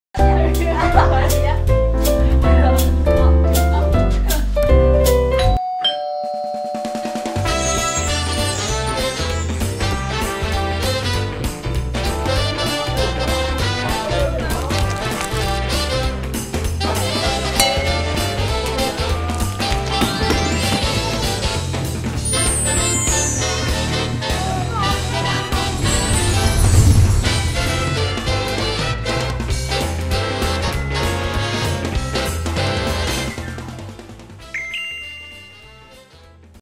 [GS25] 크리스마스 케이크
줄 서서 먹는 인생 케이크~
나만의 냉장고 APP에서 주문하세요!
당근케이크, 초코케이크, 크레이프케이크, 티라미수 케이크까지!